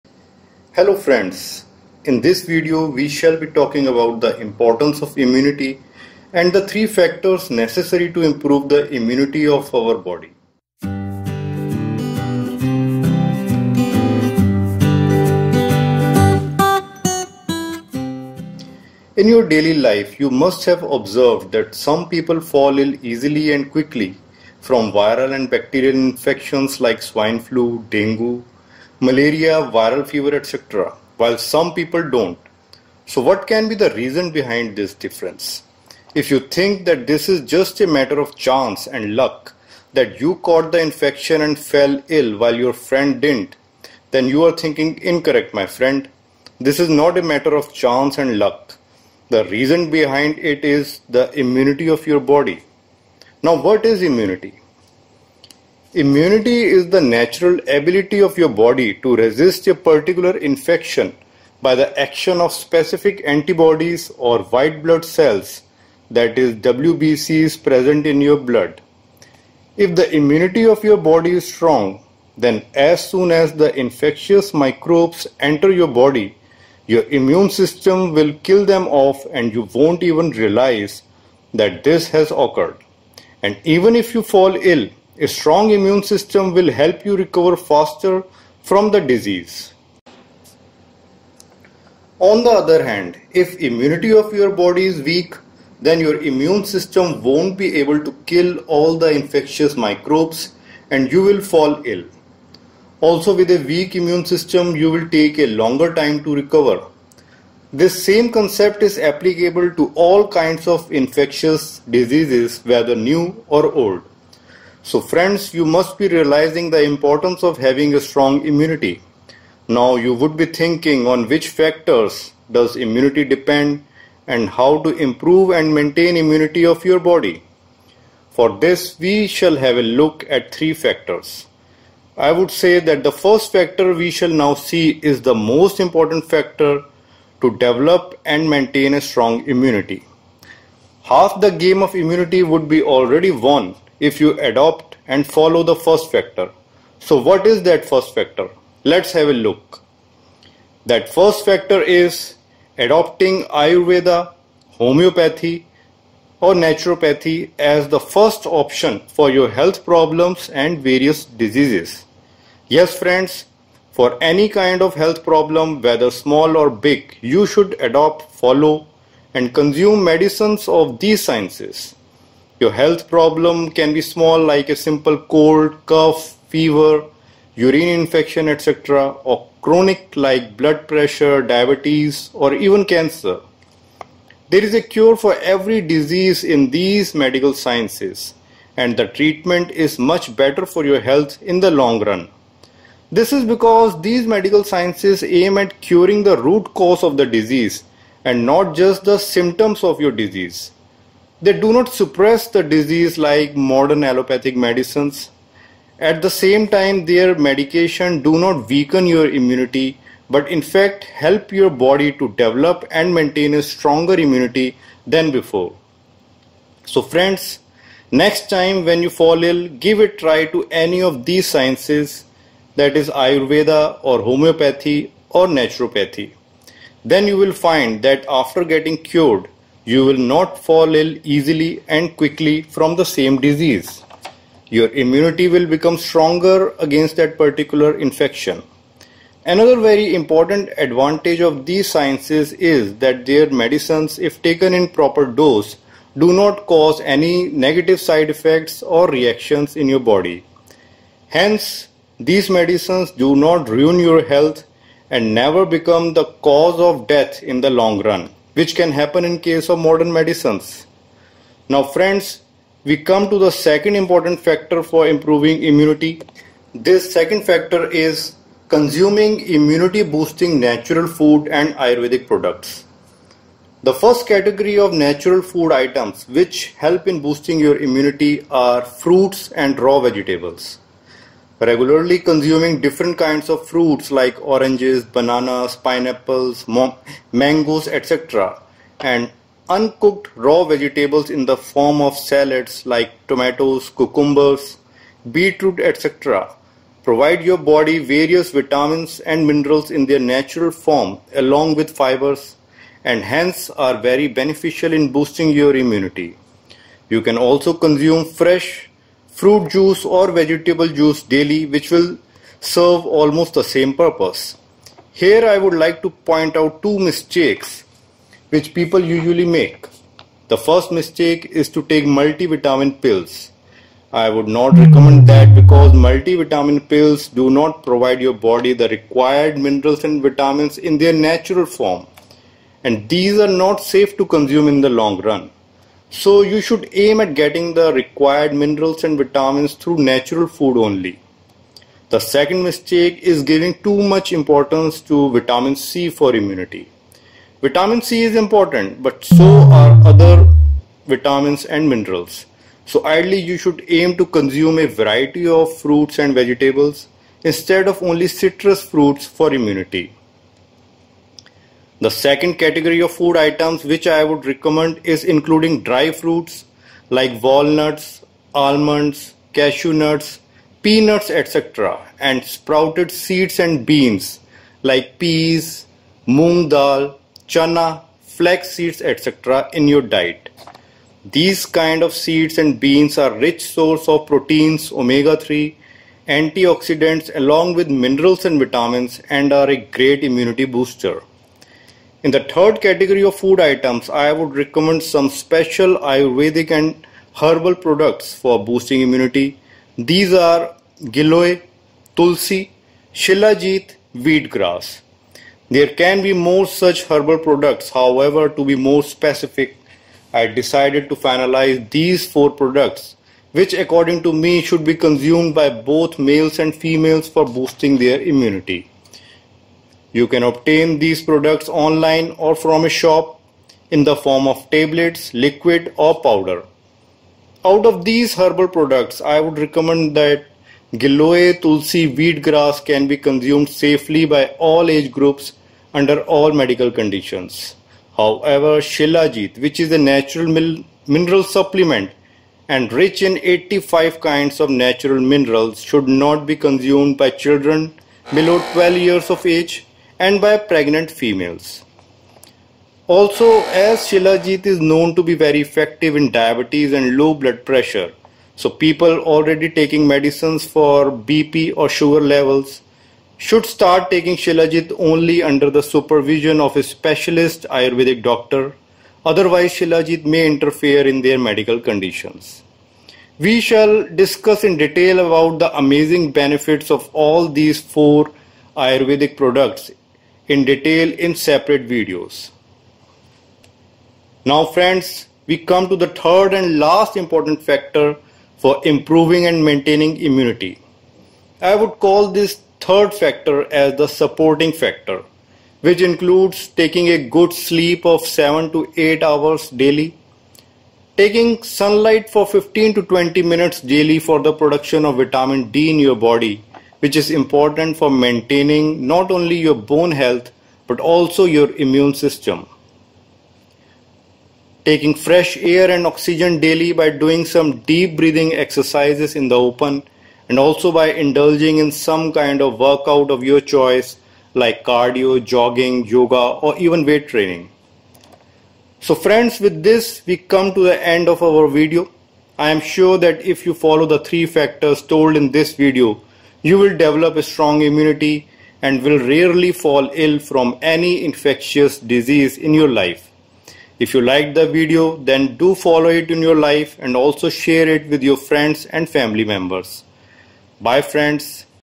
3 Factors that improve Immunity Power (In English)
Hello friends,

In this video, we analyze the importance of immunity and the 3 factors necessary to improve and maintain our immunity.

You can also see the hindi version of video on immunity, by our channel, through following link: